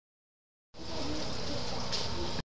David B.  jumping back crescent kick, the right way. Sorry the camera cut off the tail end.